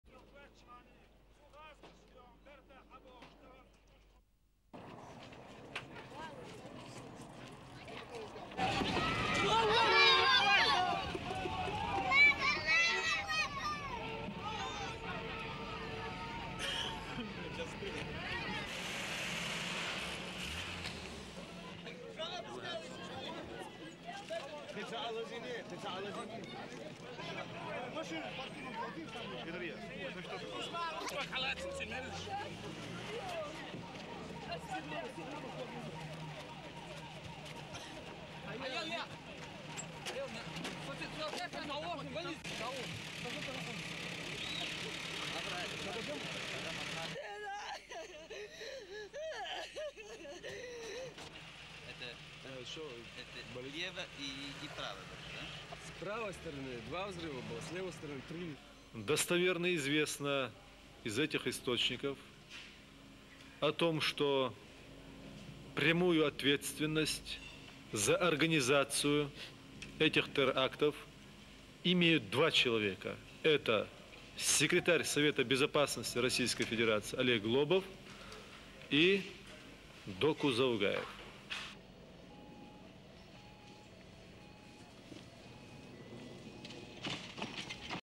RUSSIA: CHECHNYA: CHECHEN REBELS RETURNING
(11 Jun 1996) Russian/Nat

A convoy carrying Chechen rebel negotiators home from peace talks was blasted by a series of remote-control bombs Tuesday.

Seven people were reported injured in two attacks - including a Russian soldier who was accompanying the convoy.

The attacks have raised new doubts about the peace process even before Russian soldiers have begun pulling out of the rebel region.

Two sets of explosions injured seven members of a convoy of Chechen rebels and international mediators returning from negotiations with Russians.

Chechen mediators and one Russian soldier were injured in the 40 car convoy.

Gas pipes on the sides of the road severed by the blasts were  sending plumes of smoke into the air.

Representatives of the Organization for Security and Cooperation in Europe (OSCE), which is brokering peace talks, were in the convoy .

SOUNDBITE: (Russian)
What's happened? Was the explosion on the right or the left side
SUPERCAPTION: Tim Guldimann, OSCE chief negotiator

SOUNDBITE: (Russian)
Two blasts were on the right side of the road and three on the left.
SUPERCAPTION: Chechen Fighter

Rebel military chief and Chechen Information Minister  Movladi Udugov blamed the attacks southwest of the capital Grozny on the Moscow-installed regional government.

He told reporters the separatist fighters reserved the right to take adequate measures in response.

SOUNDBITE: (Russian)
Its well known from our sources that two people are directly responsible for this terrorist attack. Its the Secretary of National Security of the Russian Federation Oleg Lobov and Doku Zagaev (leader  of the Moscow-backed Chechen government)
SUPER CAPTION: Movladi Udugov, Chechen Minister of Information

The attacks came a day after the two sides reached a preliminary deal on withdrawing troops from villages in Chechnya and disarming the rebels.